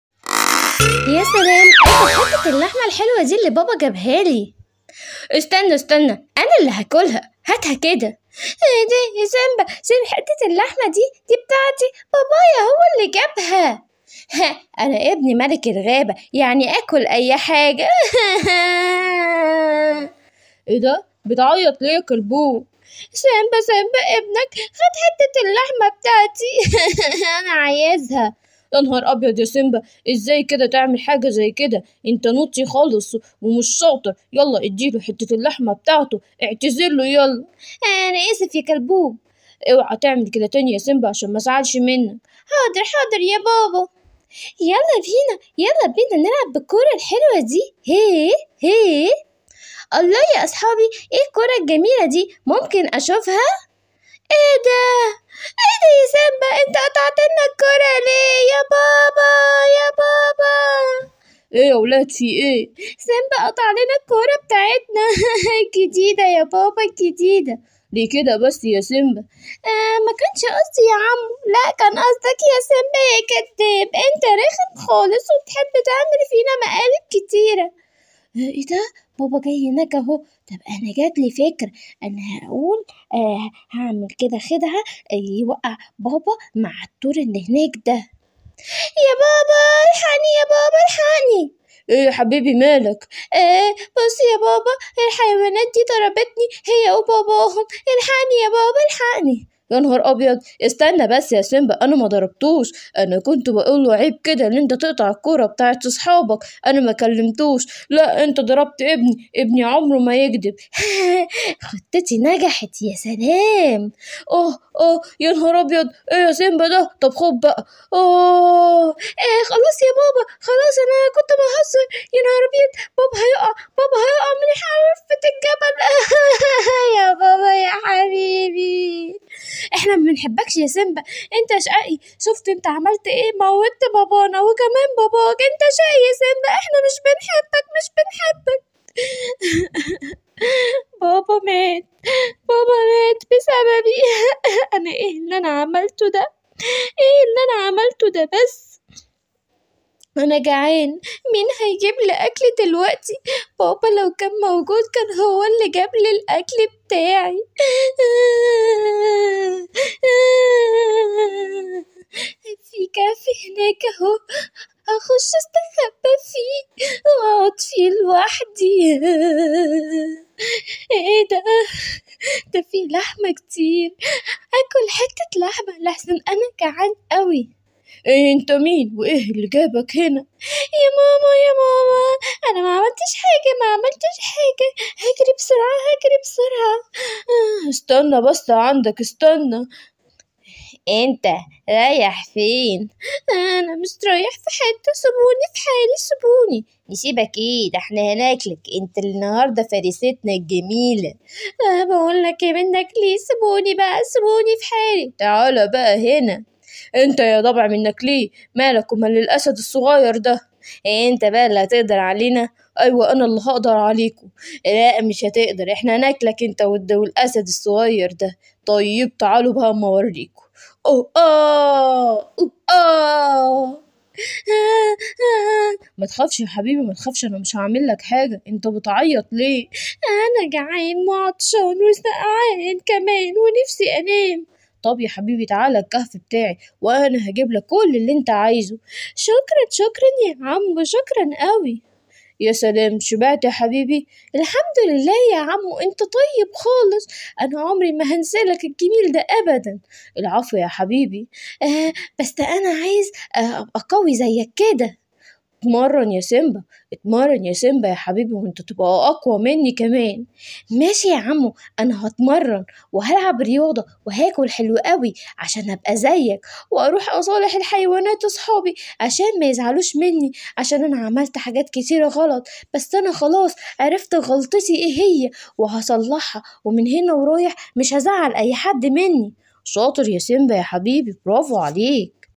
سيمبا المخادع والأسد الطيب|حكايات سيمبا|حواديت خوخه|قصص حيوانات الغابة|كرتون سيمبا ملك الغابة
مغامرات سيمبا مع حيوانات الغابة